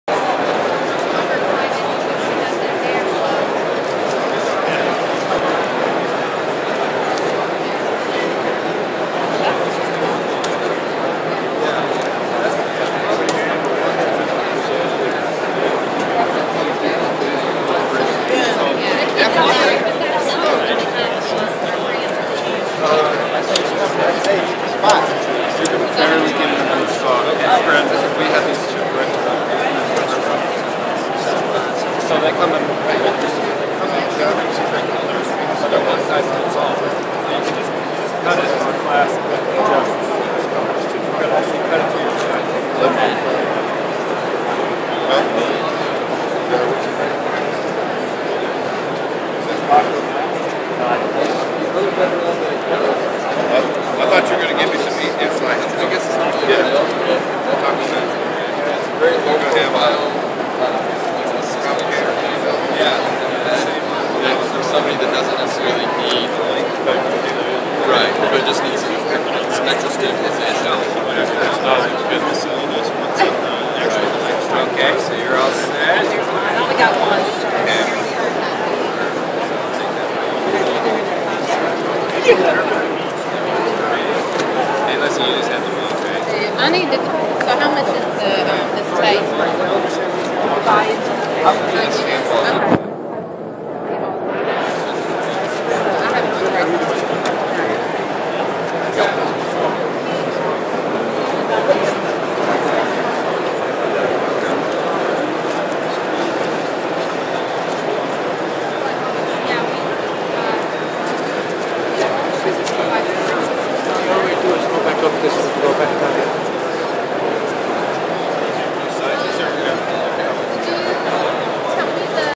Stander at Medtrade Fall 2012 Conference and Expo
Rocking and making it happen at Medtrade! The Medtrade Fall Conference and Expo is the leading event in the home medical equipment industry. It offers many exhibitors showcasing the latest home medical equipment (HME) products and services in the market.